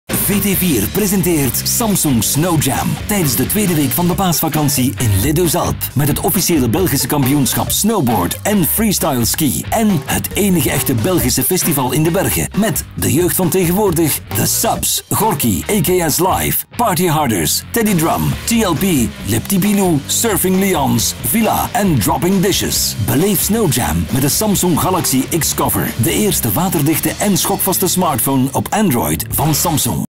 Snowjam 2012 oproepspot VT4
Belgian Championship snowboard & freestyle ski organized by GlobEvents

One of the biggest festivals in the mountaints
Music, Fun & Sports

Snowjam is a brand by GlobEvents, a travel and event company in Belgium